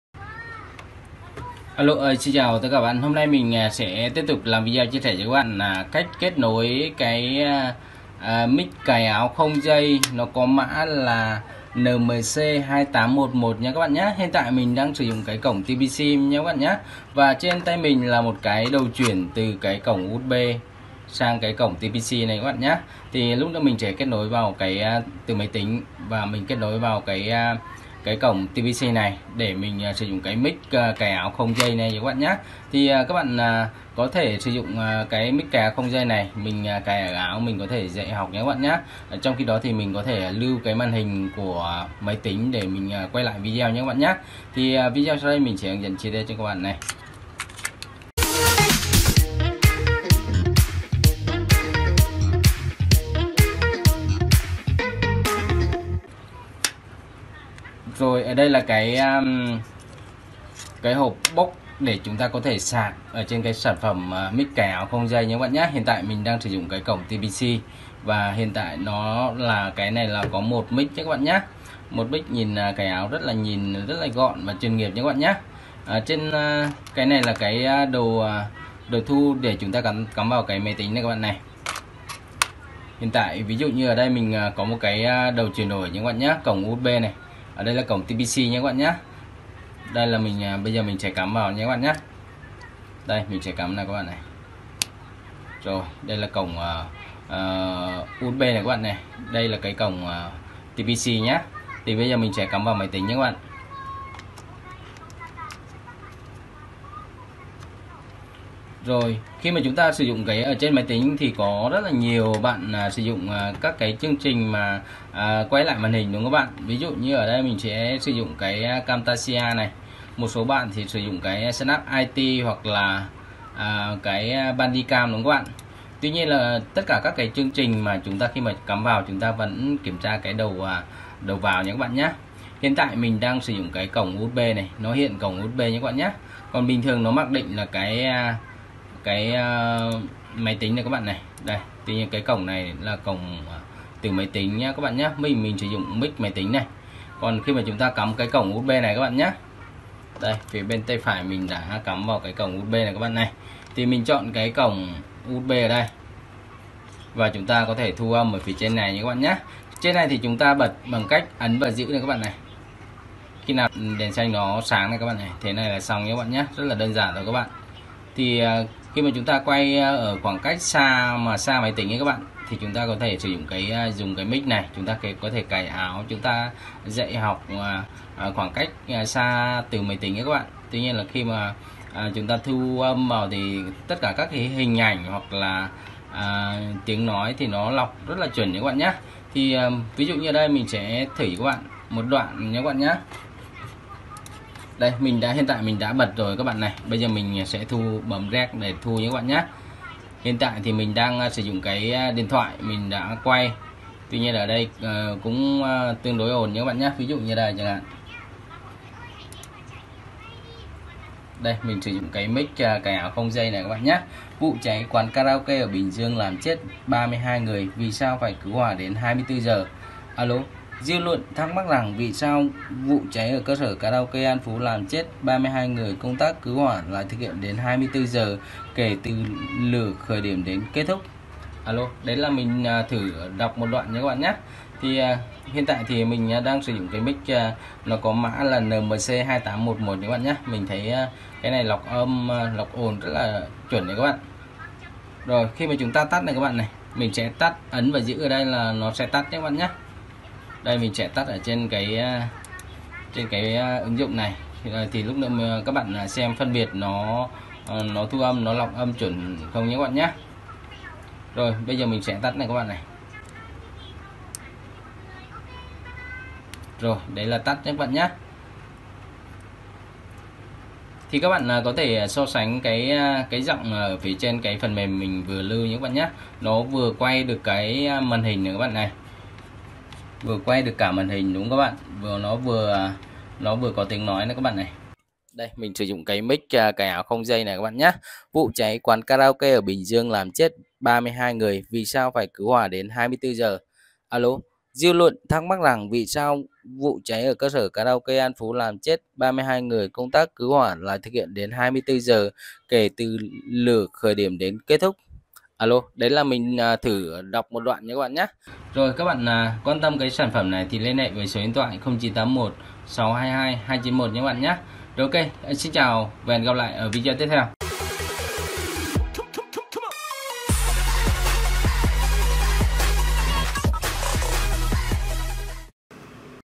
Mic thu âm cho máy tính | Mic dạy học online ưa chuộng nhất
Mic cài áo không dây không chỉ sử dụng cho điện thoại mà còn sử dụng được trên máy tính cực kỳ tiện lợi và hiệu quả
Một thiết bị không thể thiếu cho các nhà sáng tạo.
Làm video, quay tiktok, livestream, thu âm và bây giờ là thu âm cho cả máy tính...vvv
Gọn - Nhẹ - Dễ Sử Dụng
🏡 Phụ kiện Đài Trang
☎ Hotline: 0️⃣9️⃣8️⃣1️⃣6️⃣2️⃣2️⃣2️⃣9️⃣1️⃣